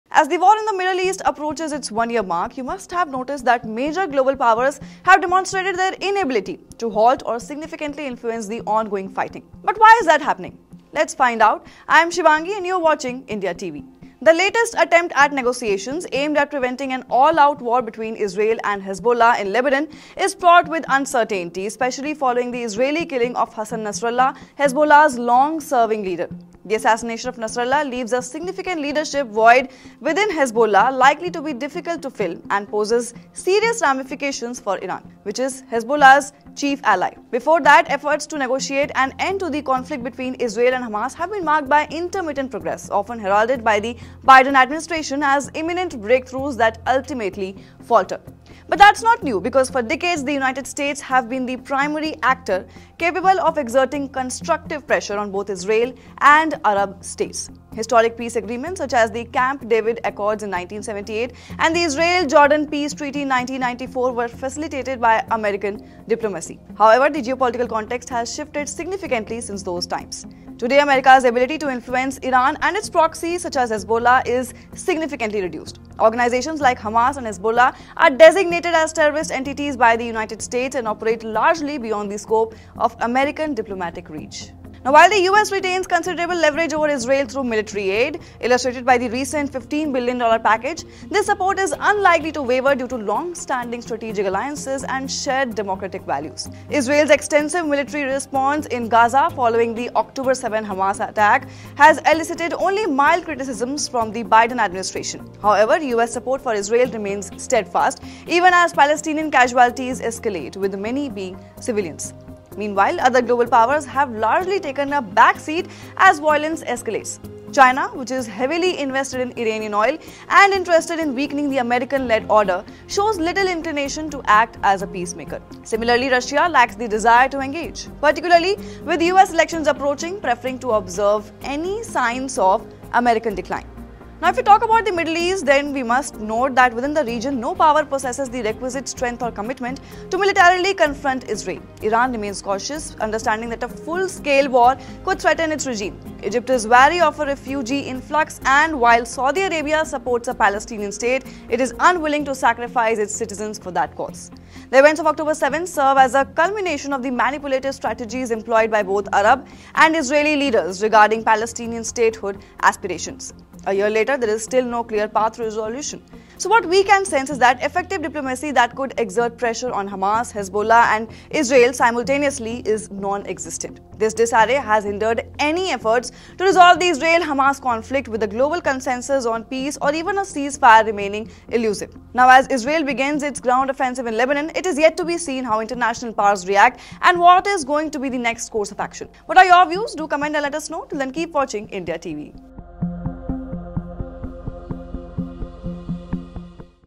Why Global Powers Struggle to Prevent Middle East Conflicts? Explained
As the Middle East conflict nears its first anniversary, major global powers have shown their inability to halt or meaningfully influence the ongoing violence. But what factors are preventing these global powers from stopping the war in this region? Watch the video to learn more.